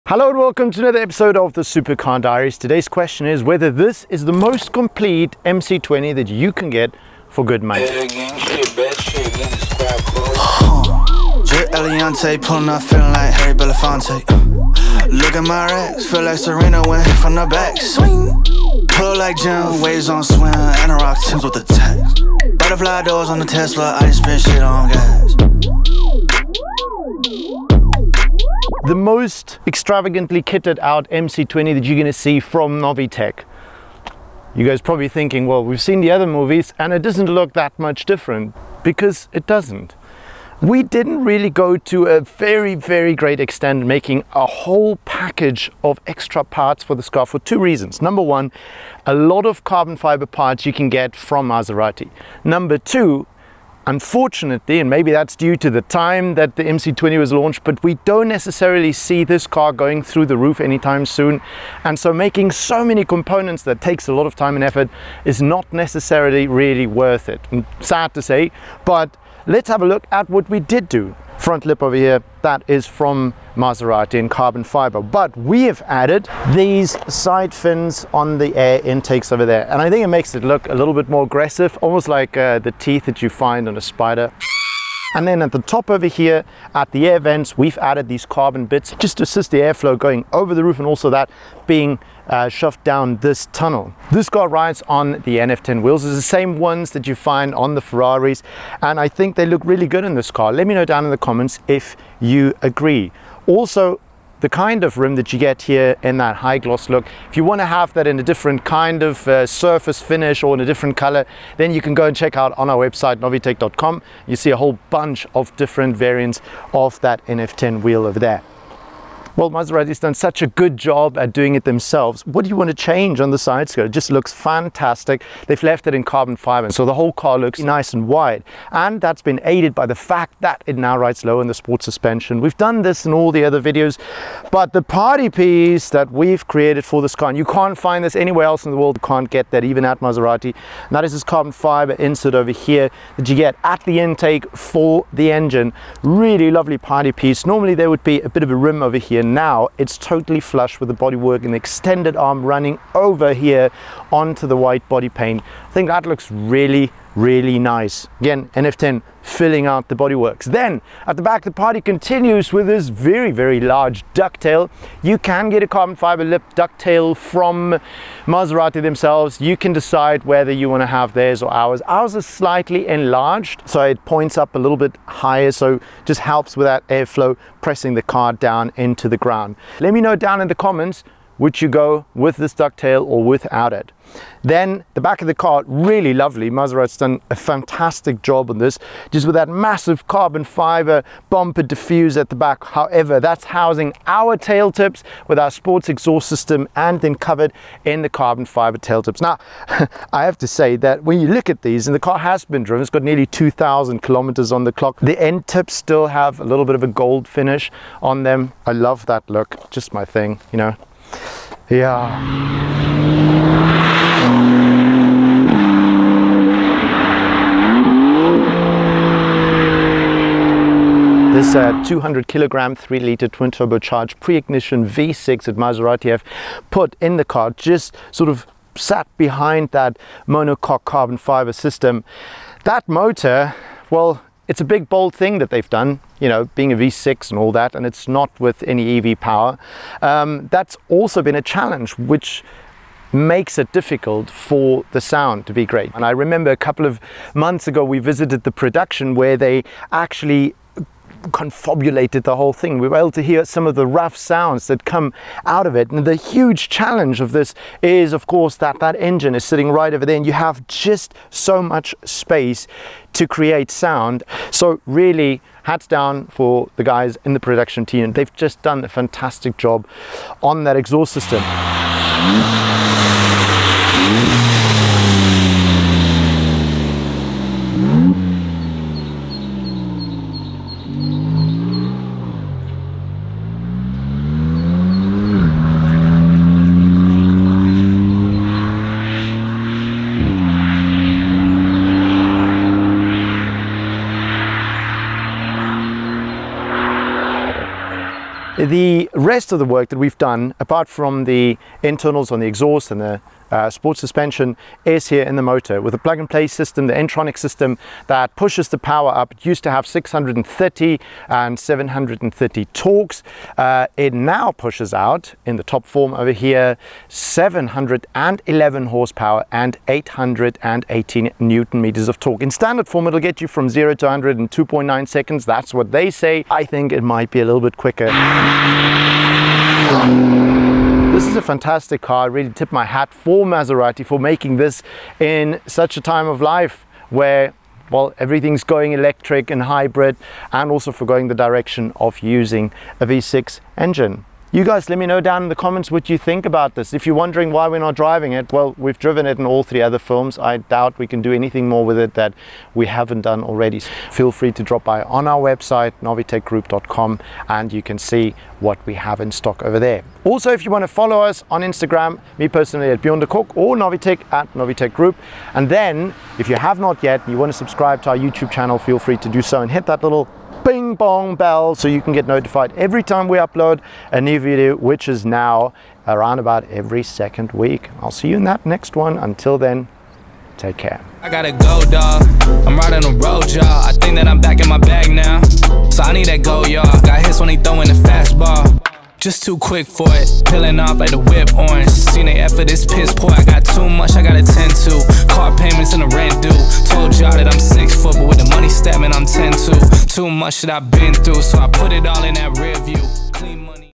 Novitec Maserati MC20, 380.000€ the most expensive Maserati 2022 / The Supercar Diaries
The Supercar Diaries / Novitec Maserati MC20

This is the most kitted out version of the MC20 that will roll from the floor at Novitec. A healthy power upgrade, louder exhaust, carbon parts, new NF10 wheels and lowered suspension make this MC20 stand out from the crowd. Did we do too little or is it just right? Have a look and let
us know in the comments.